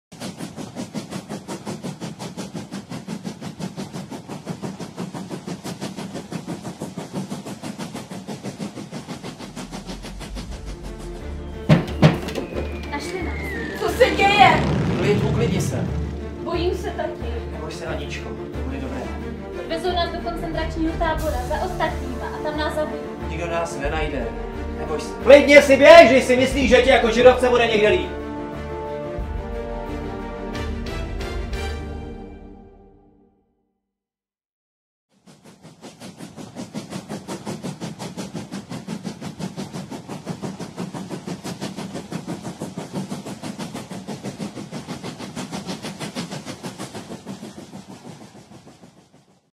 Z deníku židovky - oficiální trailer k inscenaci Divadla Exil
Druhá světová válka, pronásledování židů. Úkryt, který dává naději na přežití. Každodenní soužití v malém prostoru. Čekání na šťastný konec. Na motivy Deníku Anny M. Frankové.
hrají: Z. Dušková, V. Teplíková, R. Šťastná, L. Mucha, J. Kratochvíl, M. Zlesák; režie: Hana Moravčíková